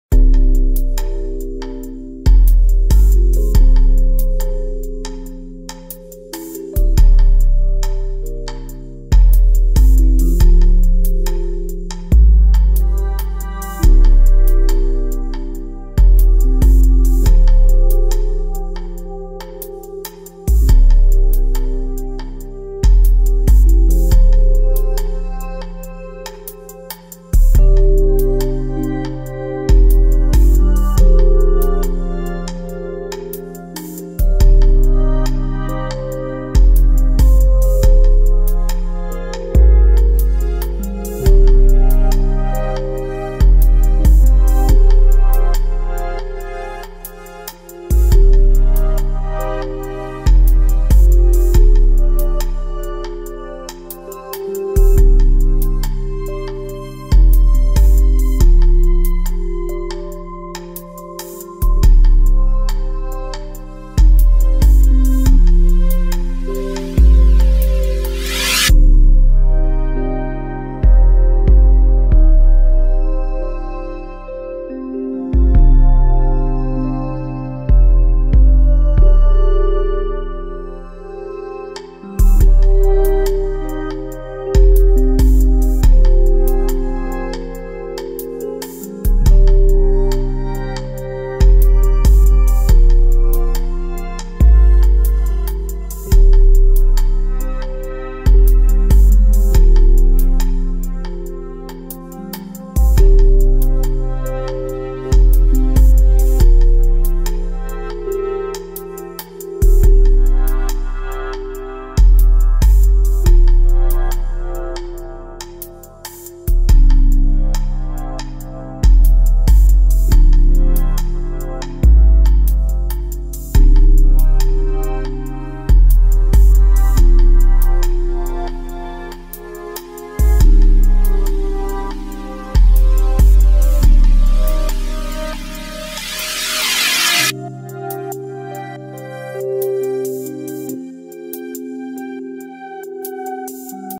Yuneec Q500- How to activate Follow Me Mode [HD]
In the video you can find out to activate the follow me function. In our opinion the follow me works well.

This Film was filmed with a Yuneec q500 and and the camera CGO2+.